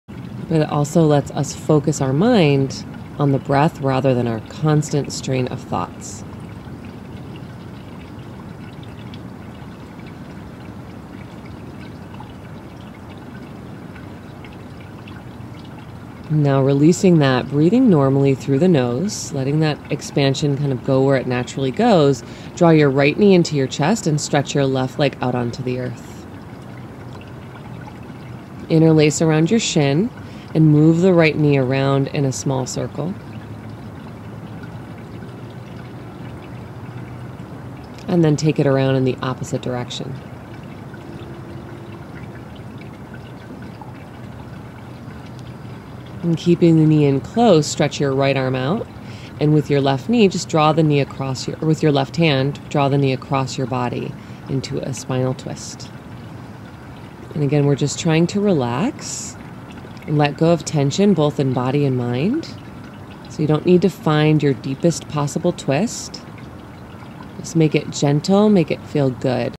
Slow Flow | Deep Sleep sample class
Sample of a 20 minute class that combines deep, calming breath exercises with gentle yoga postures to quiet the mind and relax the body.  If you lie awake at night with your mind racing, yoga before bed can be very helpful.  For the full length video go to yourBuddhi.com